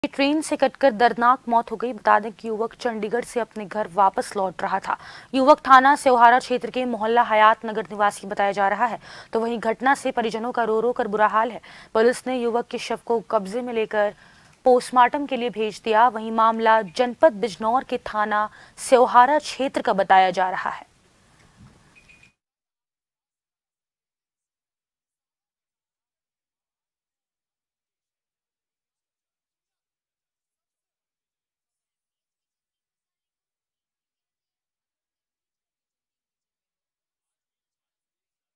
Bijnor News | ट्रेन से कटकर हुई युवक की दर्दनाक मौत, शव को कब्जे में लेकर पोस्टमार्टम के लिए भेजा
Watch JAN TV on :
Tata Play DTH : 1185
Airtel DTH: 355
JIO Fiber: 1384